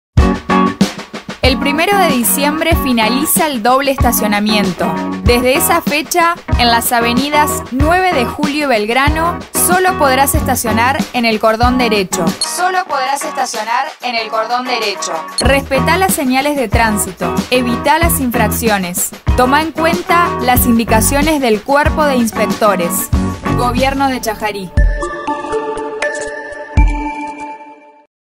Spot: Fin de doble estacionamiento
¡Mirá este spot! Desde el 1ro. de Diciembre no se podrá estacionar en la mano izquierda de las Av. 9 de Julio y Belgrano en toda su extensión. Asimismo, se habilita la doble circulación en Avenida 1º de Mayo, desde Avenida Padre Gallay hasta Avenida 28 de Mayo. Aquí, también, se prohíbe el estacionamiento en toda su extensión.